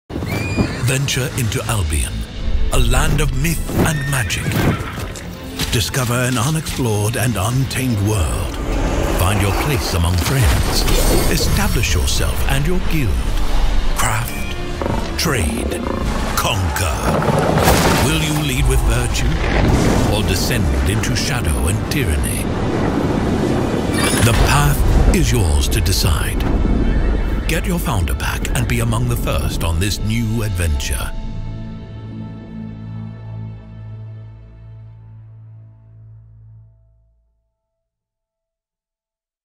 유럽 서버가 생긴다네요... 안 그래도 유저들이 그렇게 많지 않은듯 한데... / Albion Online Albion Europe Founder Packs
큰일이군요.